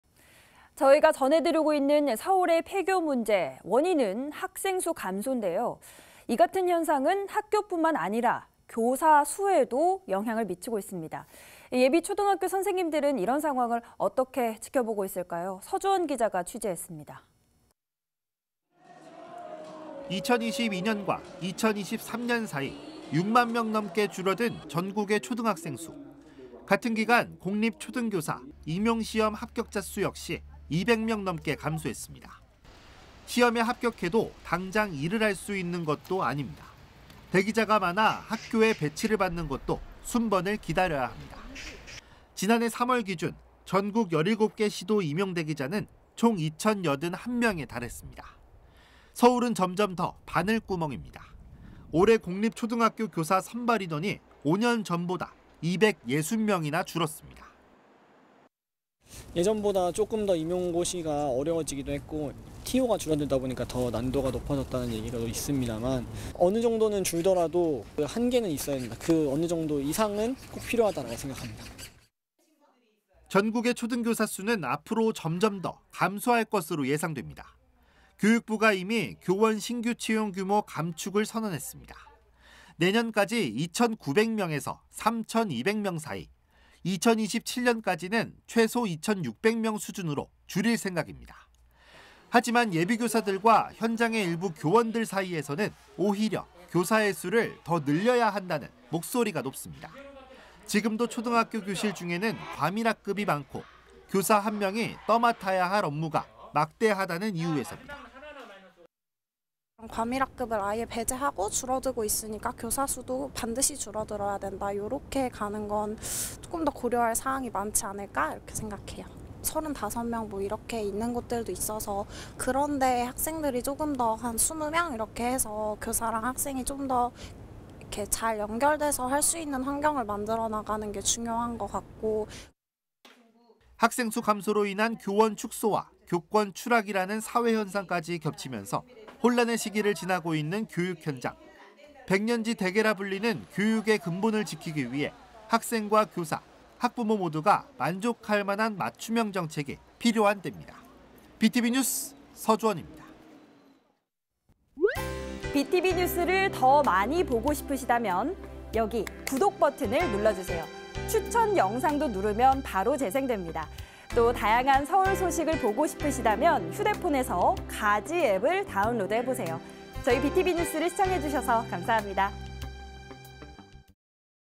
[학교가 사라진다] 학교만 사라질까?…"교사도 줄어든다"_SK broadband 서울뉴스
[B tv 서울뉴스 서주헌 기자]

[기사내용]

박종화 앵커)
저희가 전해드리고 있는
서울의 폐교 문제.
원인은 학생 수 감소인데요.
이 같은 현상은 학교뿐만 아니라
교사 수에도 영향을 미치고 있습니다.
예비 초등학교 선생님들은
이런 상황을 어떻게 보고 있을까요.
서주헌 기자가 취재했습니다.

리포트)
2022년과 2023년 사이
6만 명 넘게 줄어든 전국의 초등학생 수.

같은 기간 공립 초등 교사
임용 시험 합격자 수 역시 2백 명 넘게
감소했습니다.

시험에 합격해도 당장 일을 할 수 있는
것도 아닙니다.

대기자가 많아 학교에 배치를 받는 것도
순번을 기다려야 합니다.

지난해 3월 기준 전국 17개 시도 임용 대기자는
총 2천81명에 달했습니다.

서울은 점점 더 바늘구멍입니다.

올해 공립 초등학교 교사 선발 인원이
5년 전보다 260명이나 줄었습니다.

[서울교대 재학생]
"예전보다 조금 더 임용고시가 어려워지기도 했고 선발 인원이
줄어들다 보니까 더 난이도가 높아졌다는 이야기도 있습니다만

어느 정도는 줄더라도 그 한계는 있어야 된다.
어느 정도 이상은 꼭 필요하다고 생각합니다."

전국의 초등 교사 수는
앞으로 점점 더 감소할 것으로 예상됩니다.

교육부가 이미 교원 신규 채용 규모 감축을
선언했습니다.

내년까지 2천9백 명에서 3천2백 명 사이.

2027년까지는
최소 2천6백 명 수준으로 줄일 생각입니다.

하지만 예비 교사들과 현장의 일부 교원들 사이에서는
오히려 교사의 수를 더 늘려야 한다는 목소리가 높습니다.

지금도 초등학교 교실 중에는 과밀학급이 많고
교사 1명이 떠맡아야 할 업무가 막대하다는 이유에서입니다.

[백진영ㅣ서울교대 재학생]
"과밀학급을 아예 배제하고 (학생 수가) 줄어들고 있으니까
교사 수도 반드시 줄어들어야 된다고 하면 조금 더

고려할 사항이 많지 않을까 생각해요.
35명 있는 곳들도 있어서 그런 곳의 학생들을

20명 정도로 해서 교사랑 학생이 좀 더 잘 연결돼서 할 수 있는
환경을 만들어 나가는 게 중요한 것 같고..."

학생 수 감소로 인한 교원 축소와
교권 추락이라는 사회 현상까지 겹치면서
혼란의 시기를 지나고 있는 교육 현장.

백년지대계라 불리는 교육의 근본을 지키기 위해
학생과 교사, 학부모 모두가 만족할 만한
맞춤형 정책이 필요한 때입니다.

Btv 뉴스 서주헌입니다.

(촬영편집ㅣ최준호 기자)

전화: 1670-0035